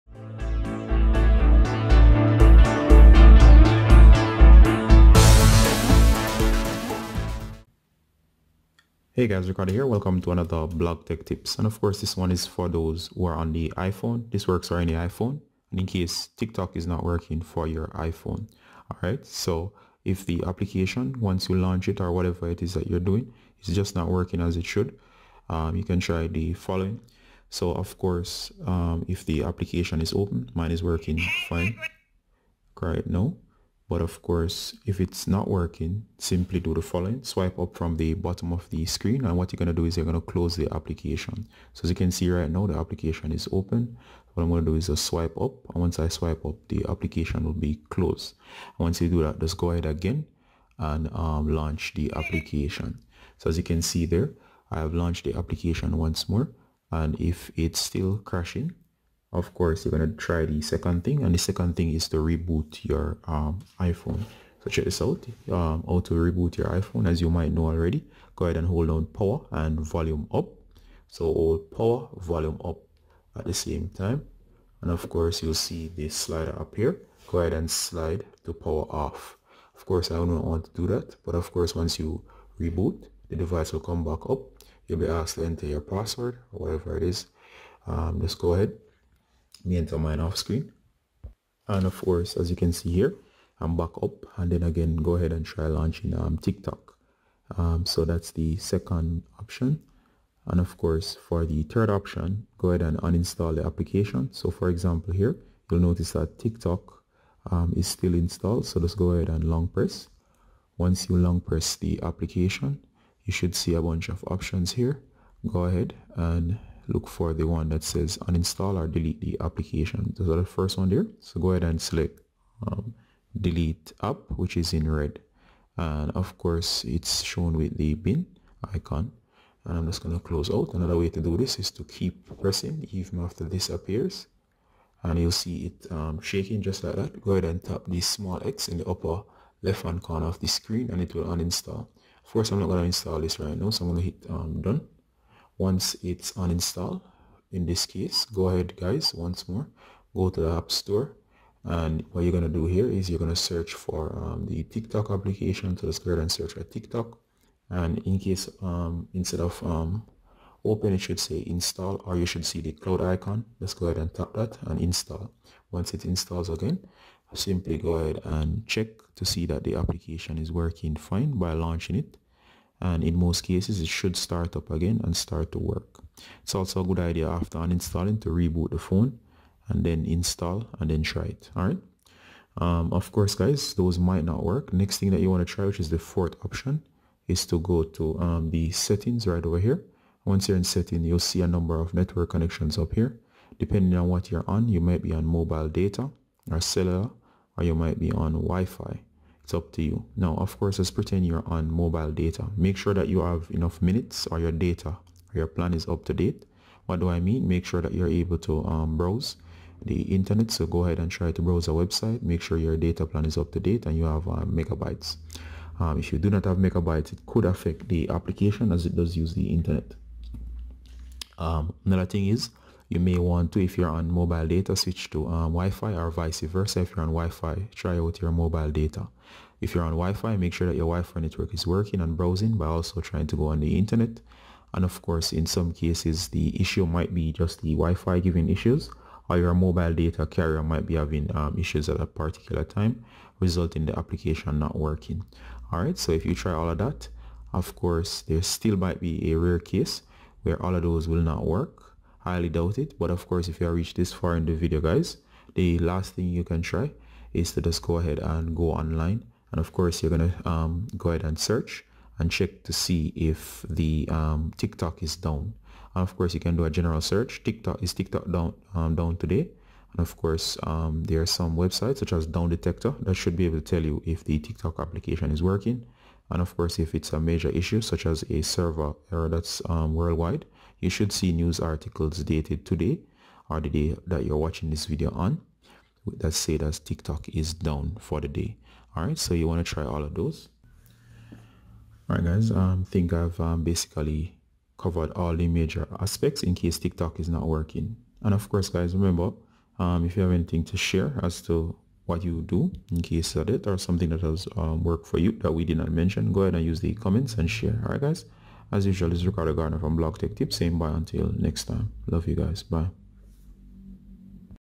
TikTok Not Working on iPhone Fix -5 Solutions
Here are Five (5) Solutions if TikTok Not working on iPhone to try. This works for all iPhone's or iOS devices. Detailed instructions on How to Fix the App in case it stop working or loading for you.